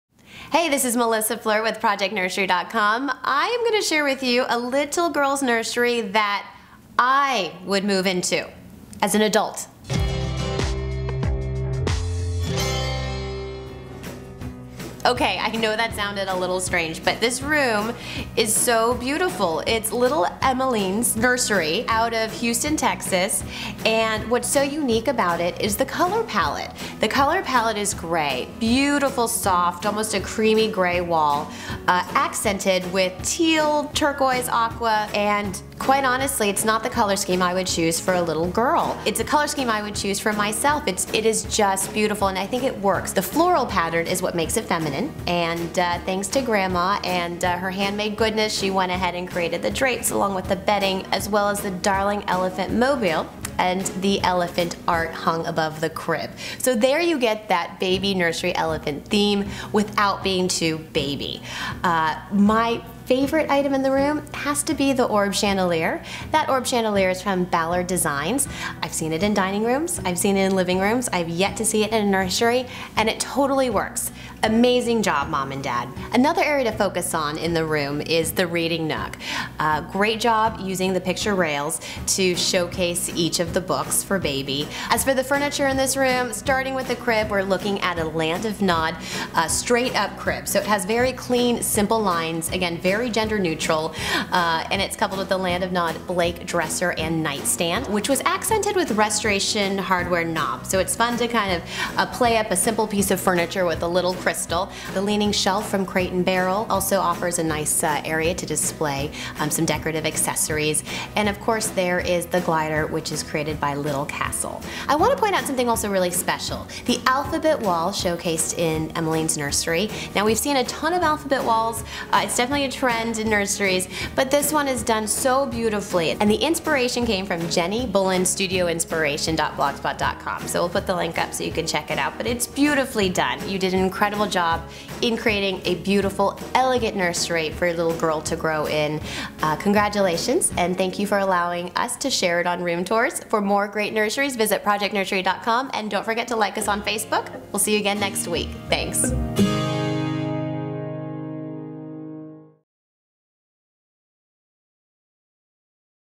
Baby Elephants in the Nursery!
Baby Emaline's nursery design is not typical of most baby girls' rooms. Instead of pink or lavender, Emaline's mom went with gray and aqua, and the effect is stunning. Key room accents include adorable elephants throughout, a wonderfully stocked book wall and a Ballard Design orb chandelier. This nursery is modern and non-traditional, but the best part is that so much of Emaline's room is homemade. The alphabet wall was crafted by Mom and Dad, and Grandma made the crib bedding, drapes and the elephant mobile and paintings. There's so much love in this room!